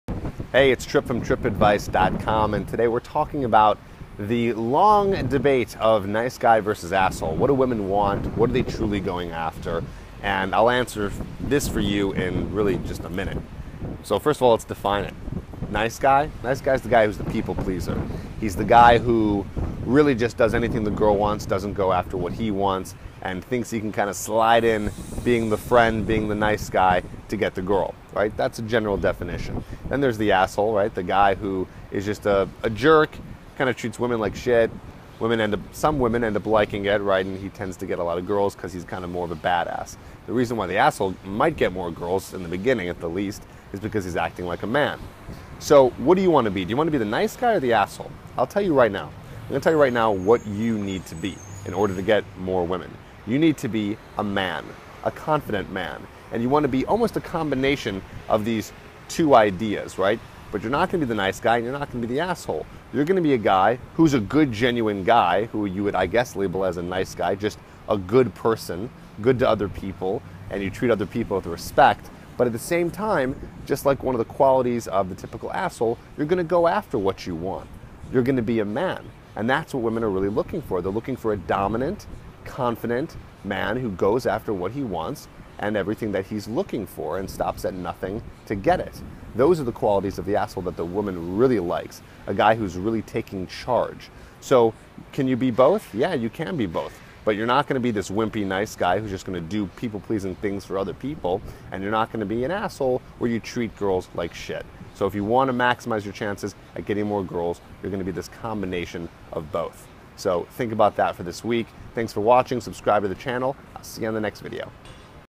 Nice Guy vs A**hole (What Women REALLY Want)
You'll learn about what a nice guy is, what an a**hole is, and what women really want.